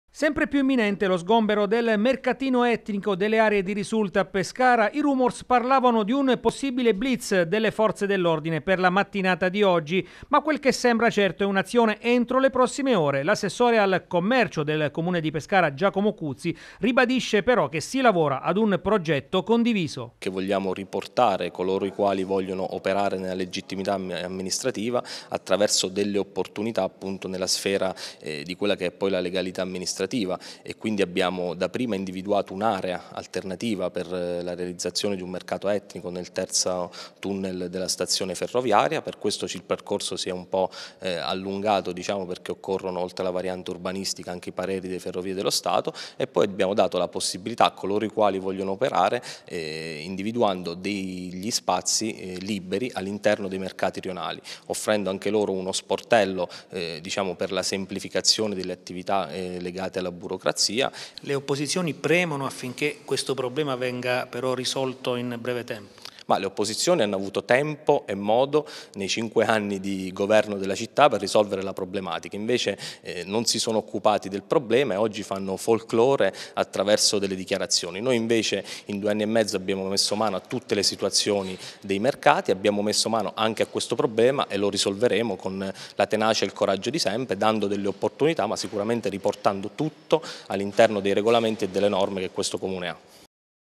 Pescara. Mercatino etnico, pronto lo sgombero
Imminente lo sgombero del mercatino etnico delle aree di risulta a Pescara. Su una questione che è diventata scottante abbiamo ascoltato l'assessore comunale al Commercio Giacomo Cuzzi